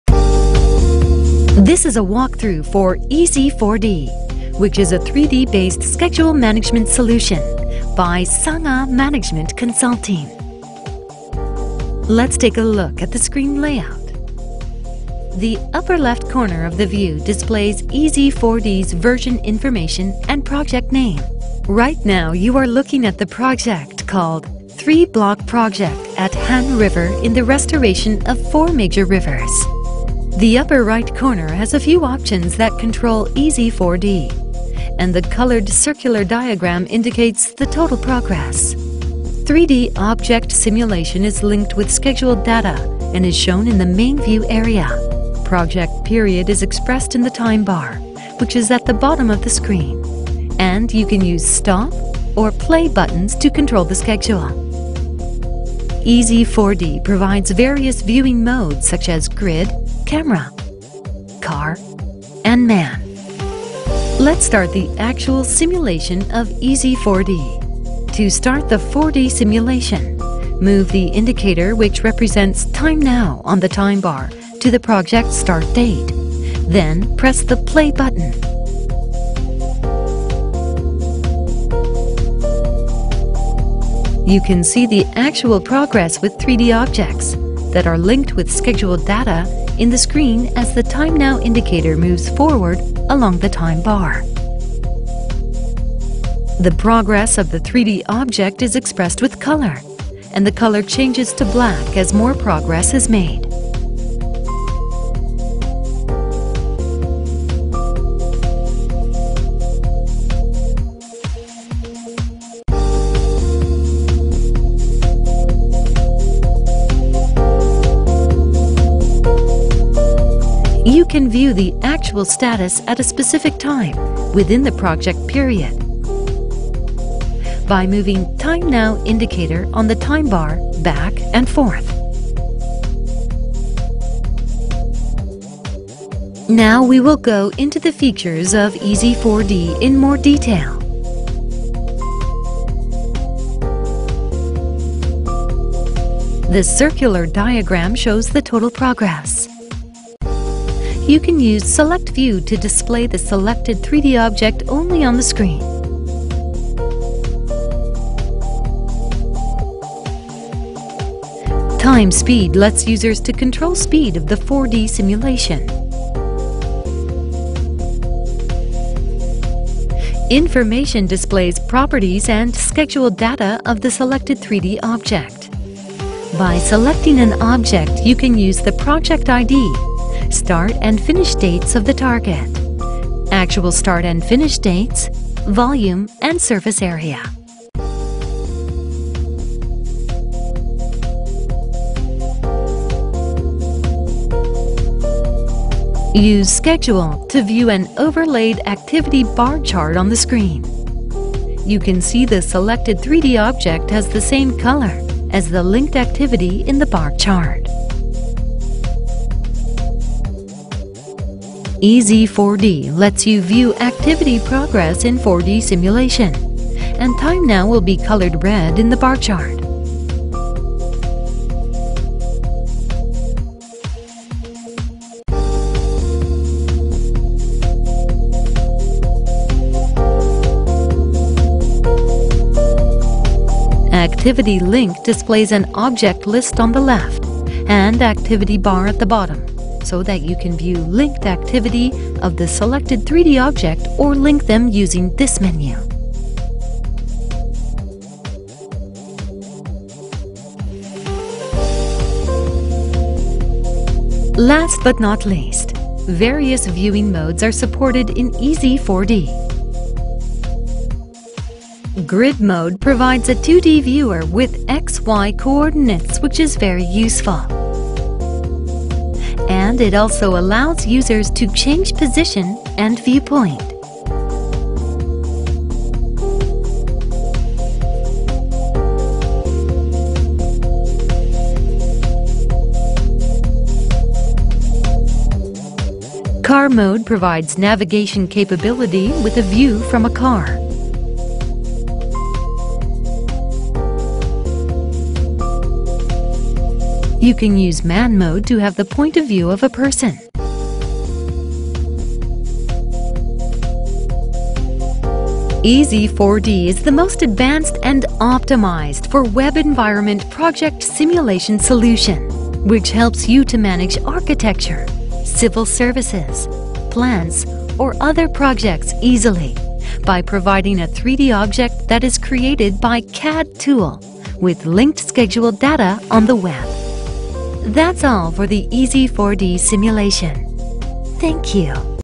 PMIS - 4D
It is a walk-through for ez-4D which is a 3D based schedule management solution by SangAh management consulting.